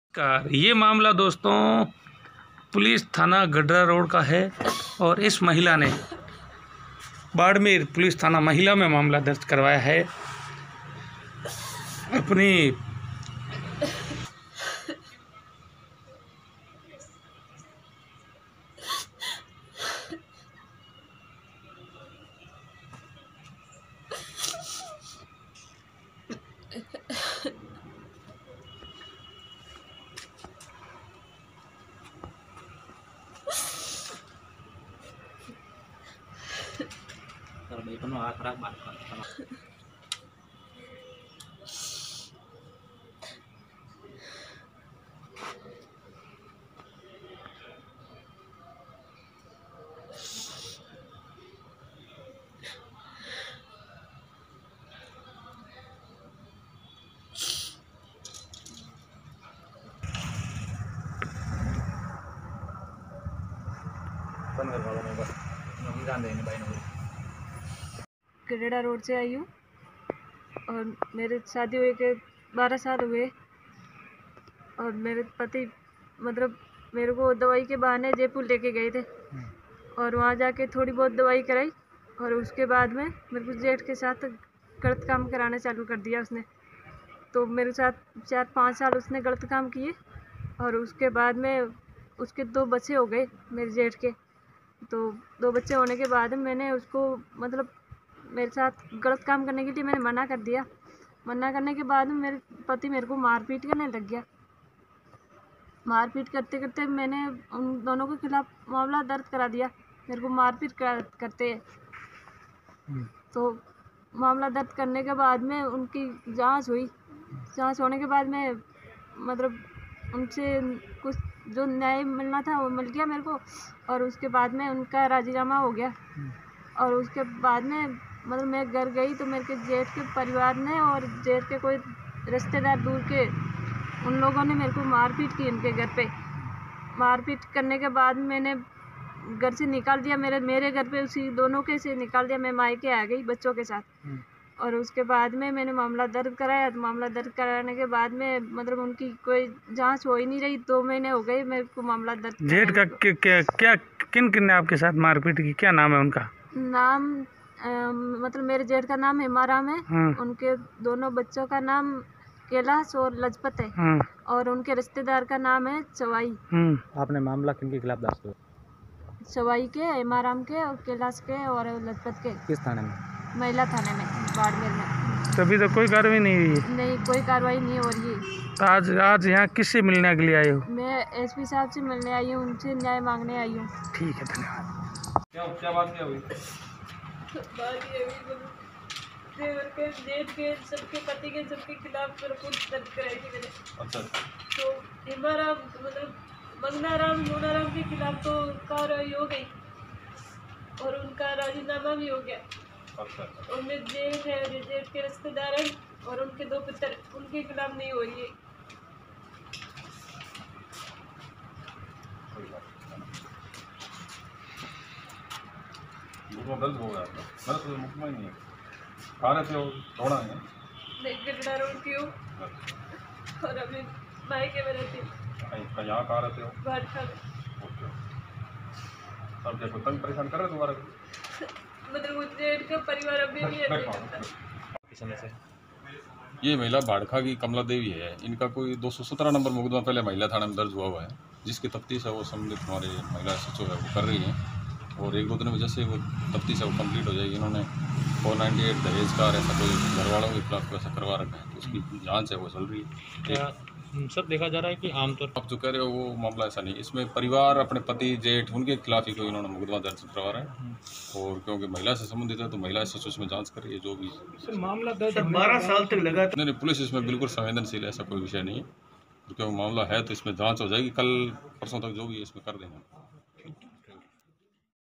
बाड़मेर। पिछले 12 साल से मेरे पति ने अपने बड़े भाई से करवाया गलत काम मेने दो बच्चों को दिया जन्म।
बाड़मेर। पिछले 12 साल से मेरे पति अपने बड़े भाई से करवाया गलत काम,दो बच्चो को दिया जन्म।
पुलिस थाना गडरारोड़ का मामला
महिला थाने में महिला ने करवाया मुकदमा दर्ज अभी तक नही हो रही हैं कार्यवाही।
परेशान महिला मिली SP आनंद शर्मा से